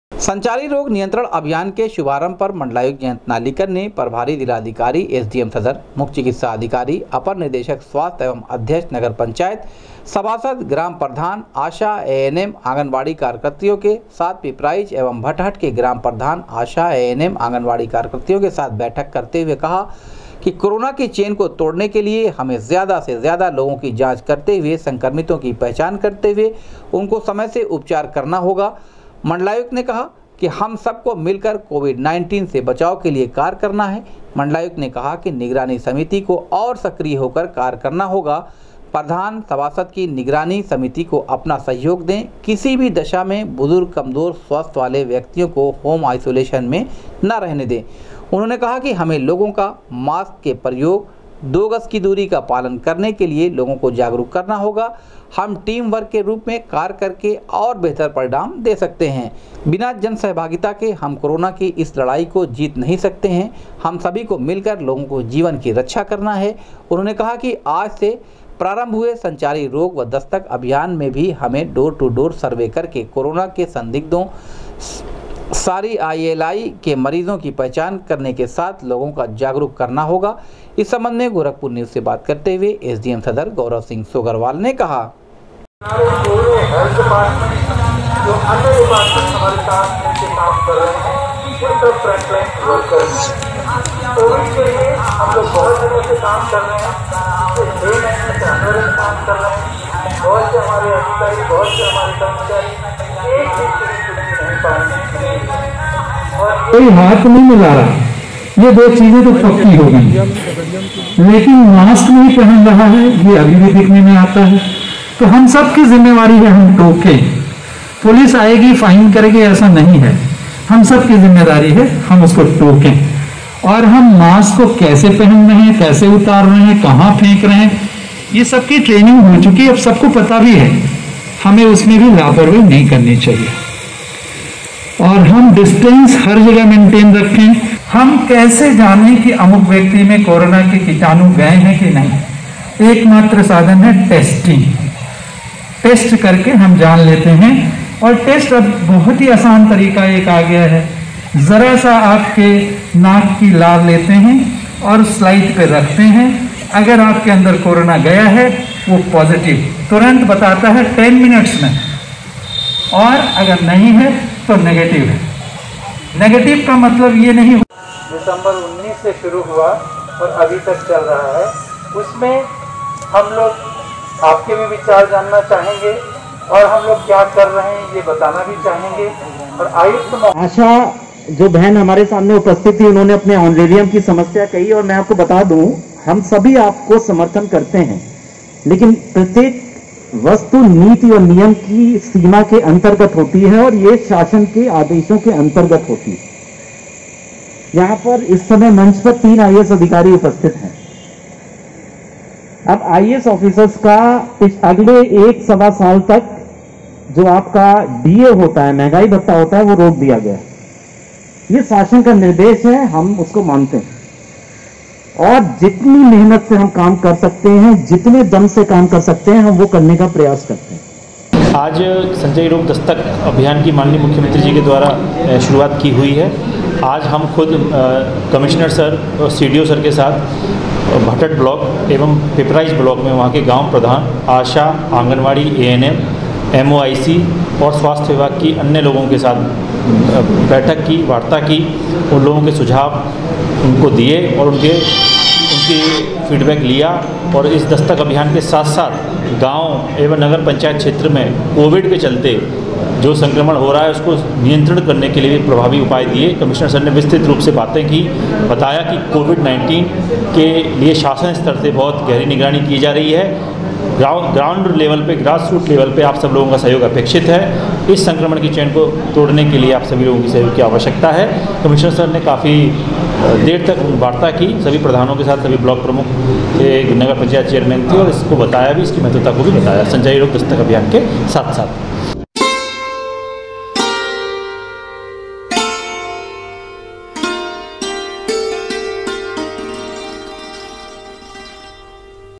संचारी रोग नियंत्रण अभियान के शुभारम्भ पर मण्डलायुक्त ने भटहट और पिपराइच में स्वास्थ्य कर्मियो के
संचारी रोग नियंत्रण अभियान के शुभारम्भ पर मण्डलायुक्त ने भटहट और पिपराइच में स्वास्थ्य कर्मियो के साथ की बैठक,कहा,संचारी रोग के साथ कोरोना संक्रमण के रोकथाम पर करे कार्य,जिससे रोका जा सके संक्रमण